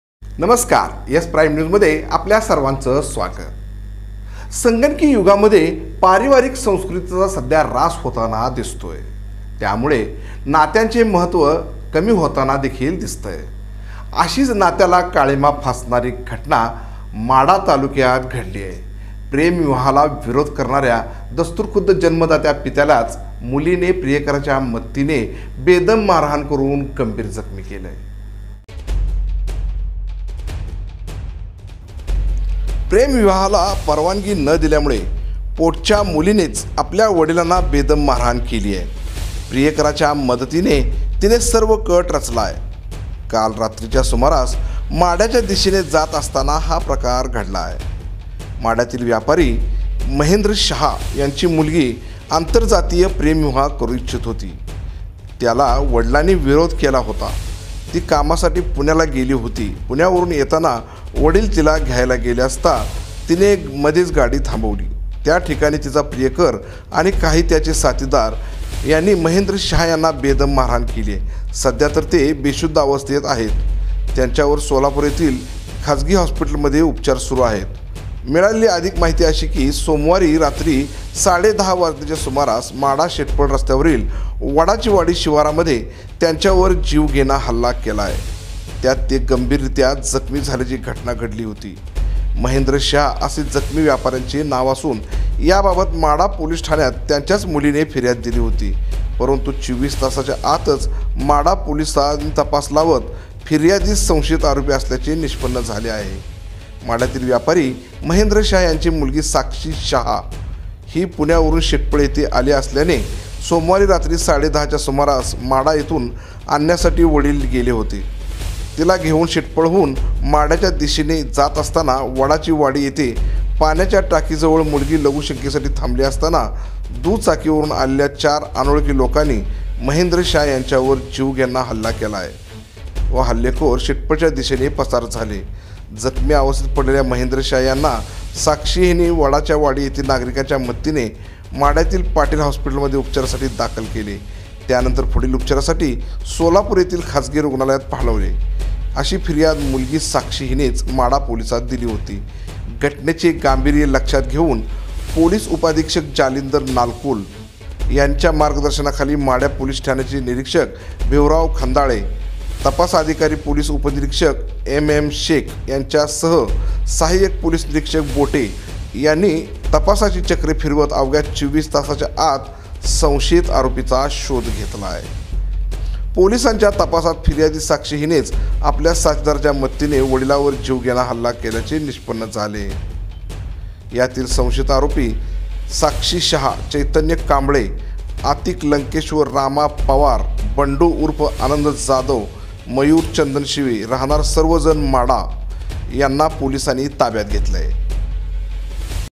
प्रेमप्रकरणातून वडिलांनाच संपविण्याचा तरुणीचा प्रयत्न | सोलापूर जिल्ह्यात नात्याला काळिमा
प्रेमप्रकरणातून वडिलांनाच संपविण्याचा तरुणीचा प्रयत्न | सोलापूर जिल्ह्यातील नातेसंबंधांना काळिमा फासणारी घटना